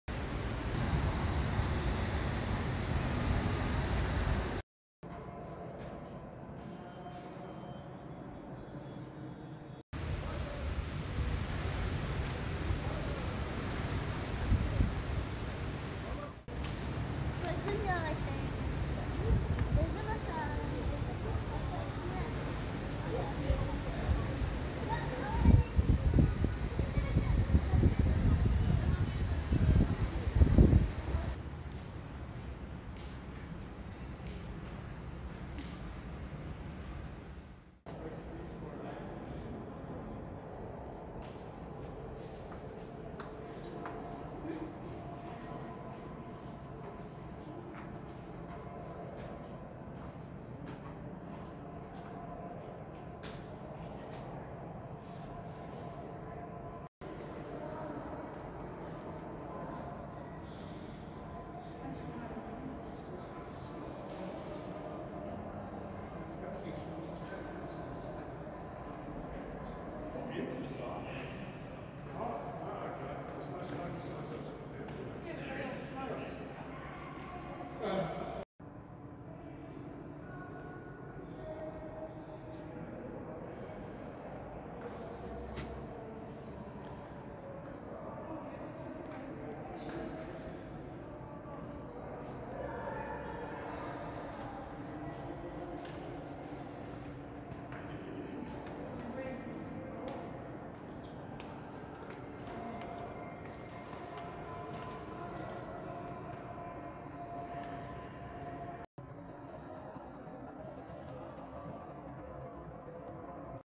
Theodosius Cistern Istanbul | Şerefiye Sarnıcı | Part #1| صهريج (خزان مياه تحت الارض) شريفية
Theodosius Cistern | Şerefiye (Sharafya) Sarnıcı | Part #1
 صهريج (خزان مياه تحت الارض)  شريفية هو واحد من أقدم الصهاريج الأرضية في منطقه السلطان احمد والذي يقع تحت مدينة إسطنبول التركية.
The Theodosius Cistern is one of many ancient cisterns of Constantinople that lie beneath the city of Istanbul, Turkey. The modern entrance is in Piyer Loti Caddesi, Fatih.
İBB Kültür A.Ş. tarafından işletilen Şerefiye Sarnıcı, 600 yıl önce yapıldığı tahmin edilen tarihin en eski sarnıcı.
Dinçer Güngörür'ün çalışmalarının yer aldığı "Taşın Perileri" heykel sergisi
Walking tour by Architect & Creative director.
Lütfen yorumlarınızı bırakın!
Much love and more videos coming!